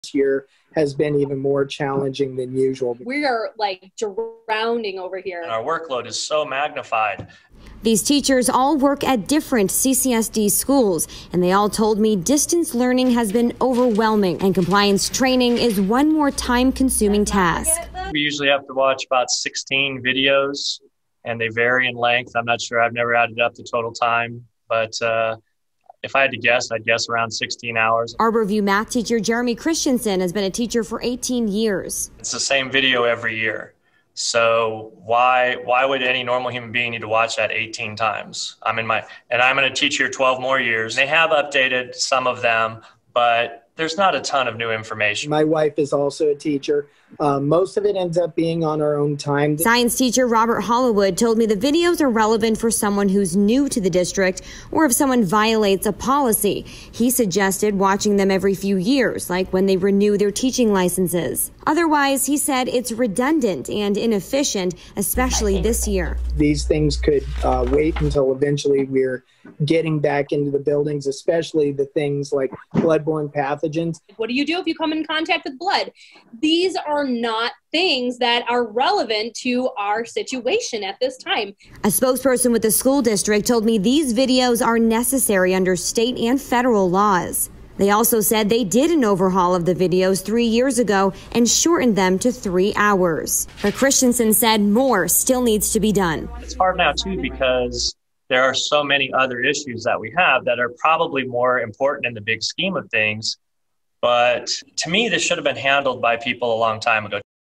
CCSD teachers call for change to compliance training
Clark County School District teachers are calling for change to compliance training, much of which, they say, isn't relevant during distance learning.